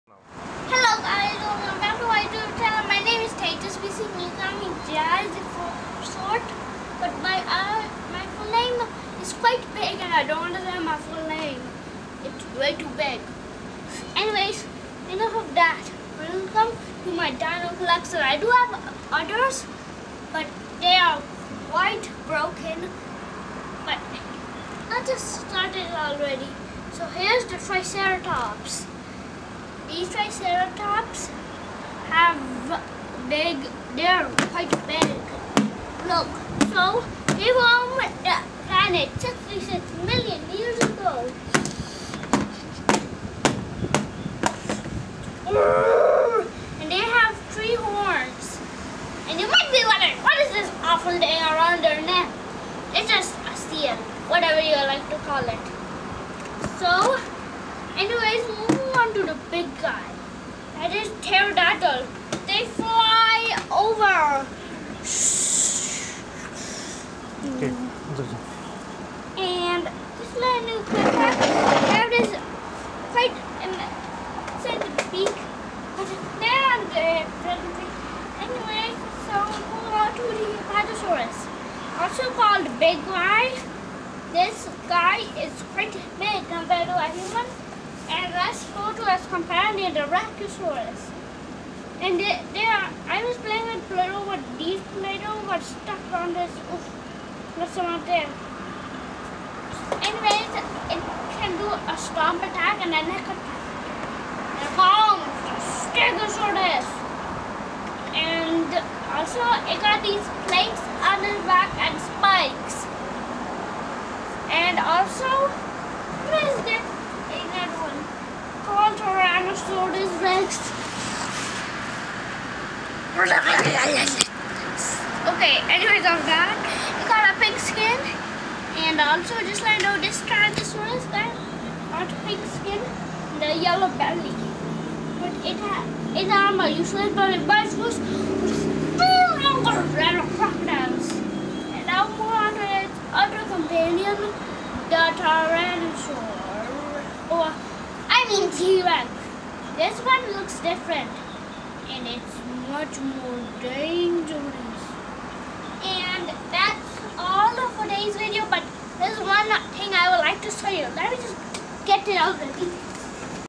My Diano Collection 1
You can ENJOY my dinosaur's top collection!
Check out my T Rex, Triceratops, Apatosaurus, Brachiosaurus, and MORE dinosaurs!